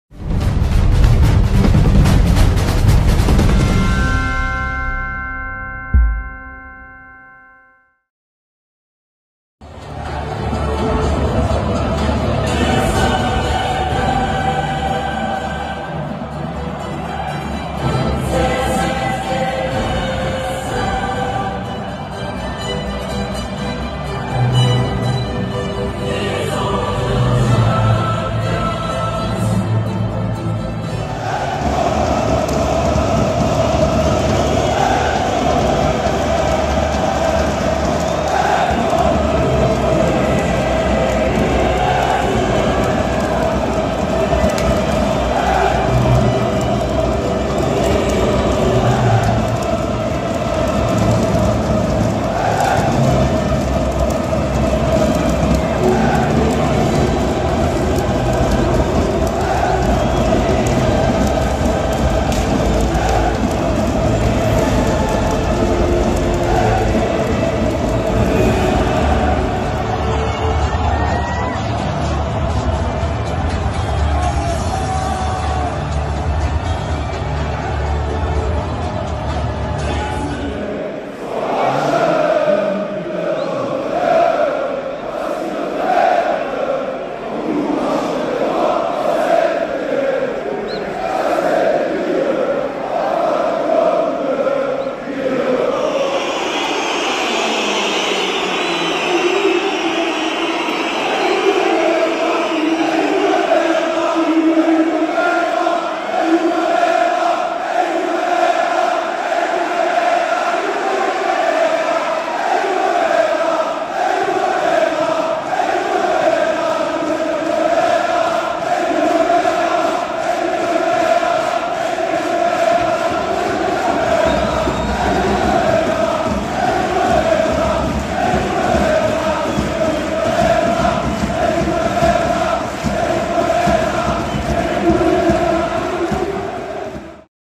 PYROSHOW INCRÍVEL DA TORCIDA DO PSG NA CHAMPIONS LEAGUE
Nesta terça (06), os ultras do Paris Saint-Germain deram show no Parc des Princes no recebimento ao time diante da Juventus, na estreia das equipes pela Champions League. Arquibancada pegando fogo com pirotecnia à vontade!

Créditos dos vídeos

@TitisduPSG (Twitter)

@PhilSANFOURCHE (Twitter

@psgcommunity_ (Twitter)

►Envie adesivos da sua torcida ou presentes:
Rua João Ramalho, 1512 - AP 81 | CEP: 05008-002 | São Paulo - SP
A/C - Adelino Martins